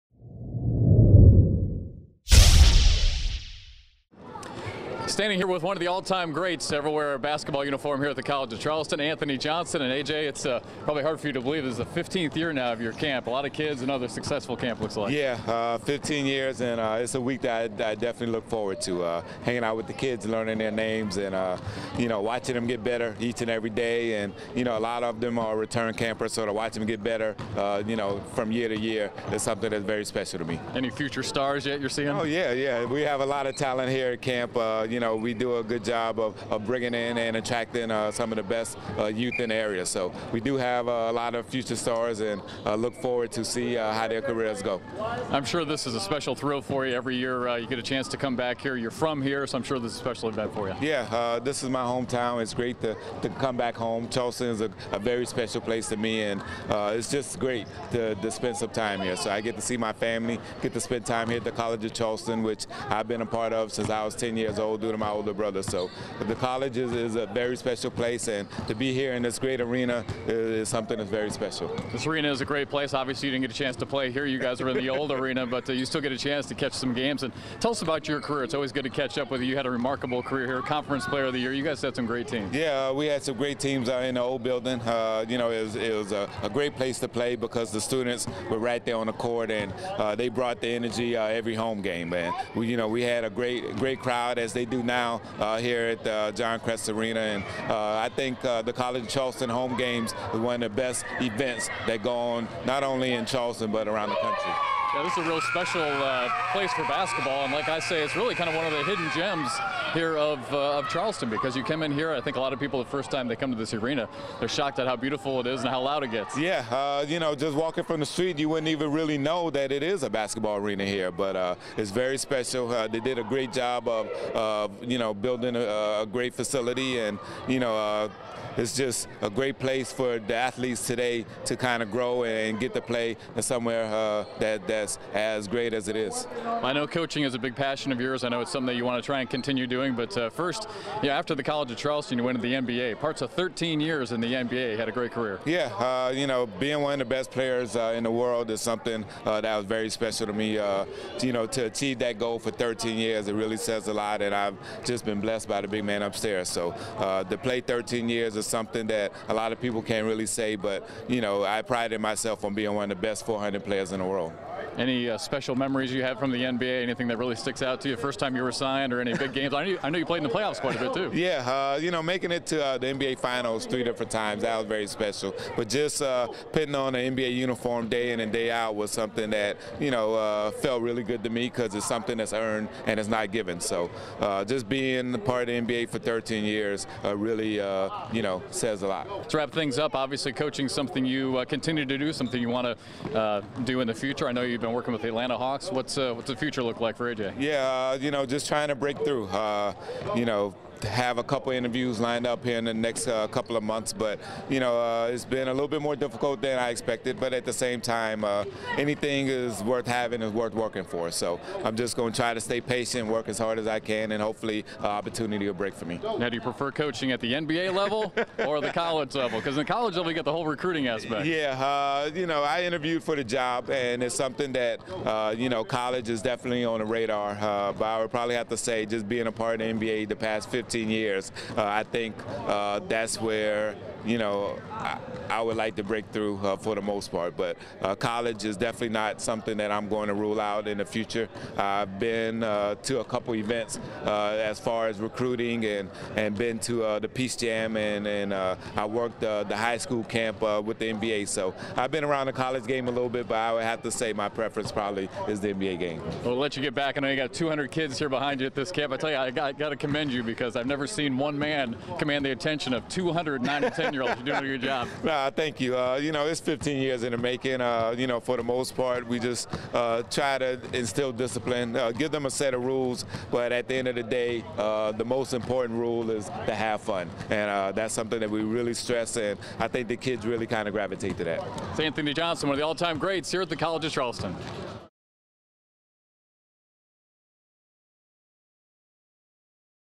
8.6.12 Anthony Johnson One-On-One Interview
Jeff McCarragher talks to College of Charleston great Anthony Johnson about a variety of subjects including his annual basketball camp, playing in the NBA and his aspiring coaching career.